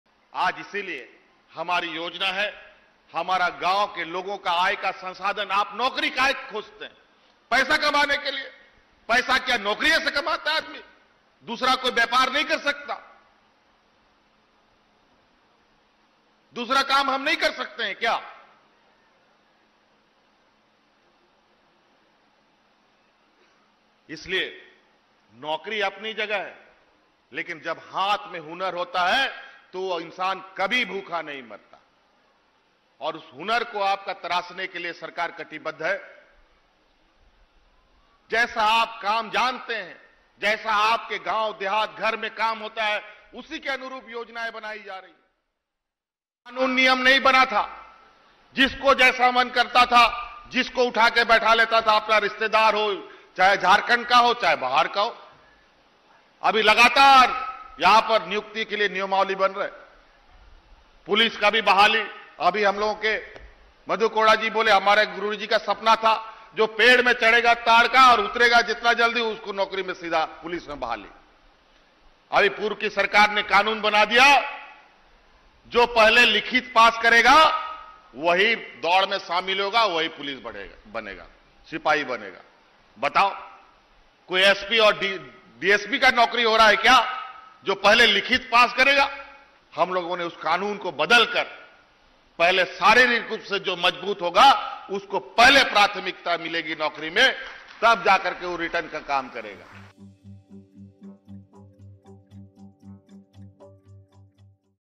Jharkhand के CM हेमंत सोरेन का अजीबो-गरीब बयान, बोले- युव नौकरी क्यों खोजते हैं ?, व्यापार करें
Jharkhand के मुख्यमंत्री हेमंत सोरेन के बयान पर सियासत शुरू हो गई है. चाईबासा में एक कार्यक्रम के दौरान सोरेन ने कहा कि युवा नौकरी क्यों खोजते हैं उन्हें व्यापार करना चाहिए लेकिन वो युवाओं को नौकरी देने के अपने वादे पर कुछ नही बोले. देखिये सोरेन ने क्या कहा